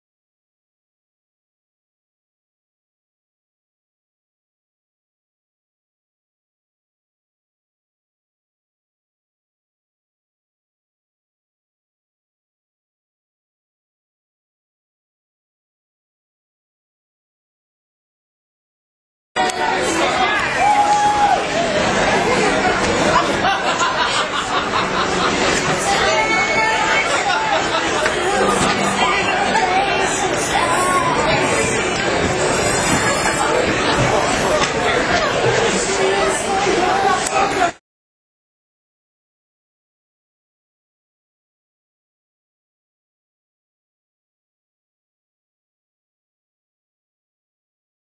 Balloons
Cruise ship passengers being kept busy